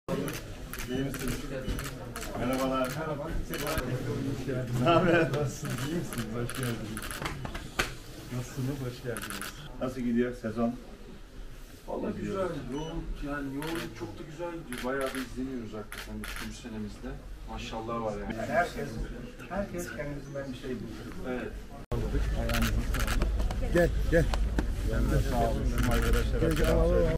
Foreign Minister Çavuşoğlu visited the set of the TV series Gönül Dağı
Foreign Minister Mevlüt Çavuşoğlu visited the set of TRT's "Gönül Dağı" series, which is watched with interest.
Çavuşoğlu visited the set of the TV series "Gönül Dağı" in Sivrihisar district of Eskişehir, where he came within the scope of a program, and met with the series crew and actors.